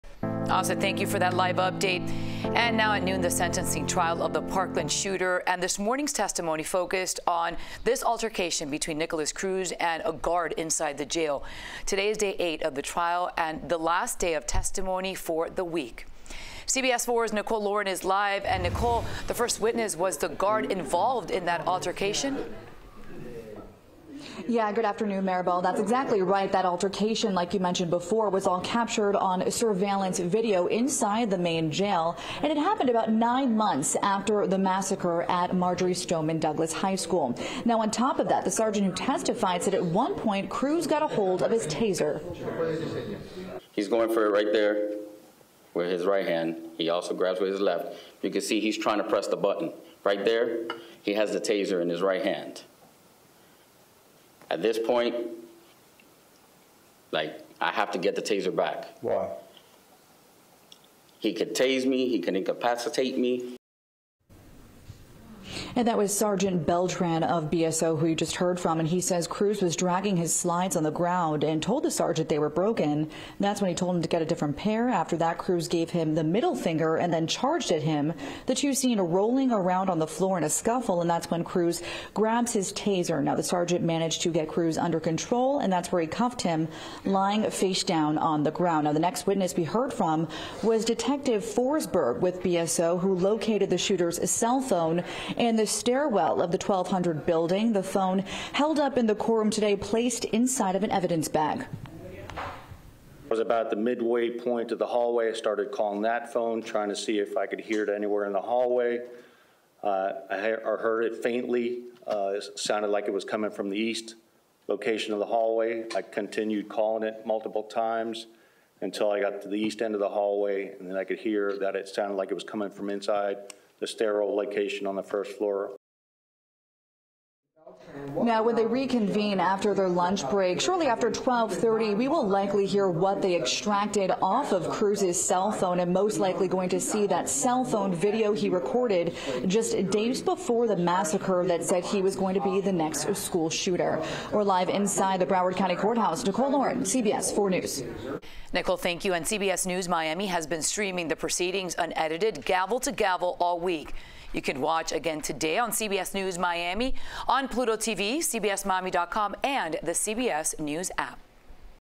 Footage Of Fight Between Nikolas Cruz And Officer In Jail At Center Of Early Day 8 Testimony
Prosecution showed footage of Parkland school shooter Nikolas Cruz fighting with a corrections officer during Day 8 of the sentencing trial.